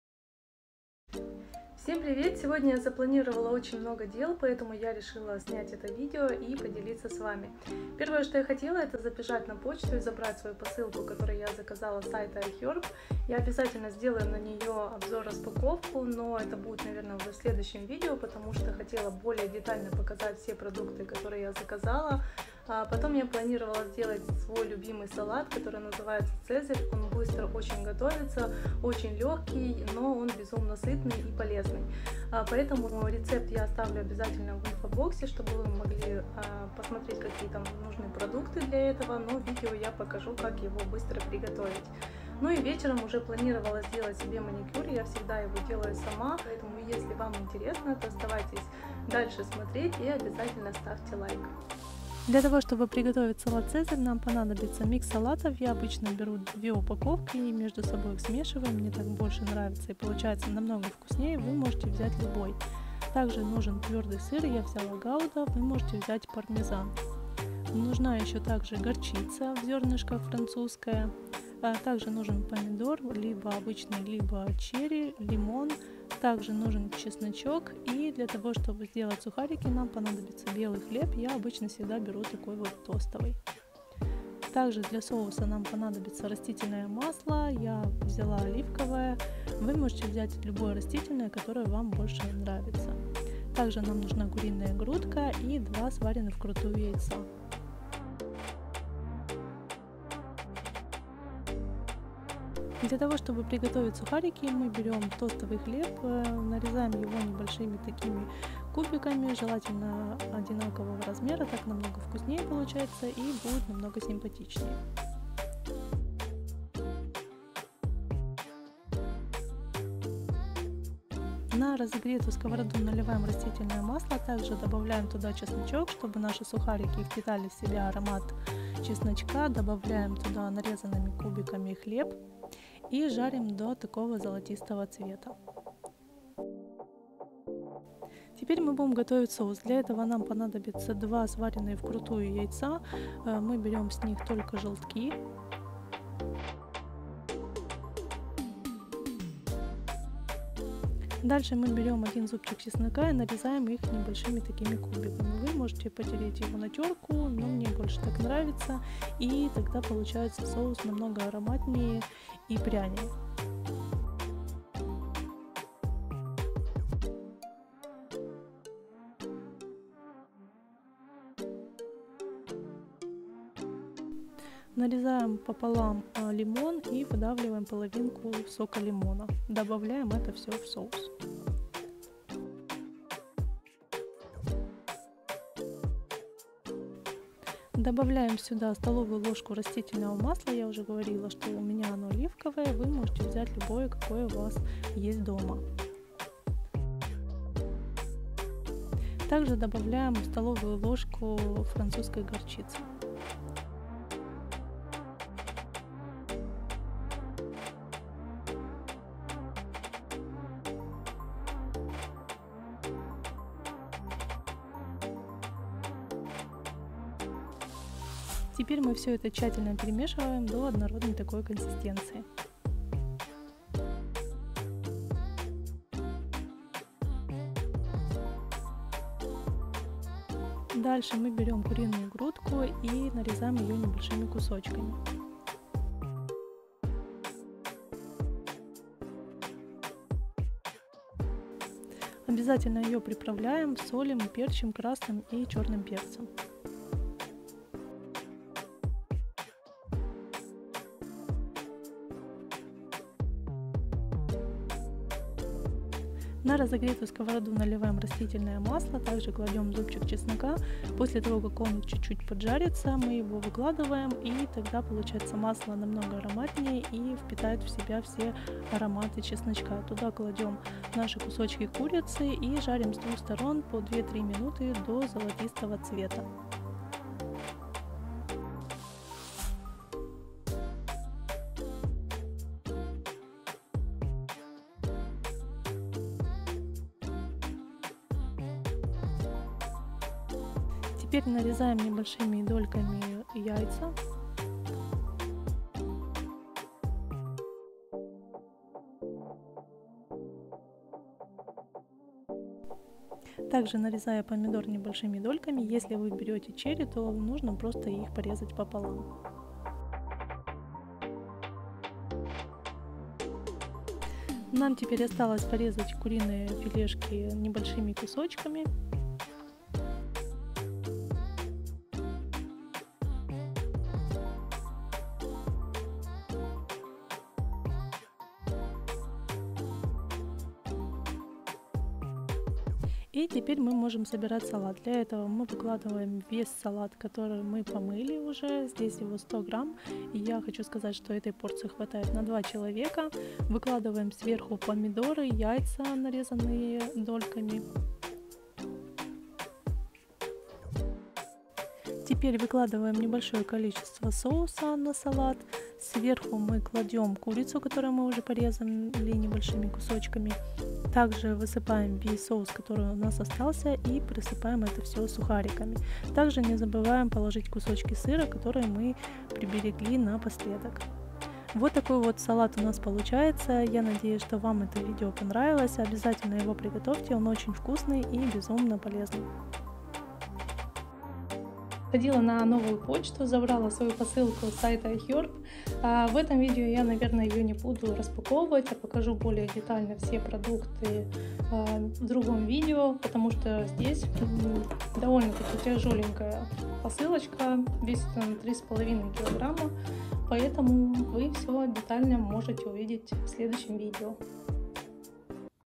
ГОТОВИМ САЛАТ ЦЕЗАРЬ ❤️ | VLOG | РАСПАКОВКА IHERB ❤️
Сегодня я покажу как я быстро и вкусно приготовить салат 🥬 «Цезарь»

Понадобиться:
Микс салата 100-120 гр.
Куриная грудка 1 шт.
Оливковое масло.
Горчица французская.
Хлеб нарезной 4 кусочка.
Чеснок 3 шт.
Сыр твёрдый Гауда или Пармезан для украшения.

Музыкант: Jef